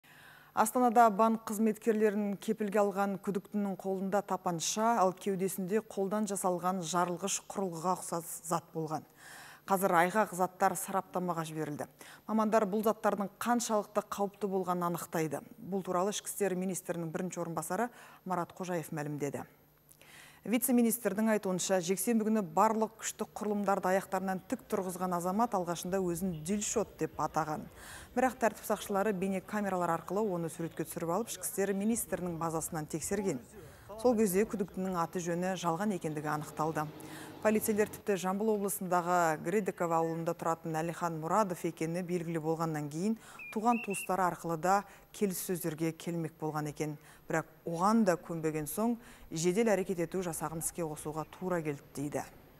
Банк қызметкерлерін кепілге алған күдіктінің кеудесінде жарылғыш құрылғыға ұқсас зат болған
Астанада банк қызметкерлерін кепілге алған күдіктінің қолында тапанша, ал кеудесінде қолдан жасалған жарылғыш құрылғыға ұқсас зат болған. Қазір айғақ заттар сараптамға жіберілді. Мамандар бұл заттардың қаншалықты қауіпті болғанын анықтайды. Бұл туралы ішкі істер министрінің бірінші орынбасары Марат Қожаев мәлімдеді.
Вице-министрдің айтуынша, жексенбі күні барлық күштік құрылымдарды аяқтарынан тік тұрғызған азамат алғашында өзін Дильшот деп атаған. Бірақ, тәртіп сақшылары бейнекамералар арқылы оны суретке түсіріп алып, ішкі істер министрінің базасынан тексерген. Сол кезде күдіктінің аты-жөні жалған екендігі анықталған. Полицейлер тіпті Жамбыл облысындағы Гродеково ауылында тұратын Әлихан Мұрадов екені белгілі болғаннан кейін, туған-туыстары арқылы да келіссөздерге келмек болған екен. Бірақ оған да көнбеген соң жедел әрекет ету жасаған іске қосуға тура келді дейді.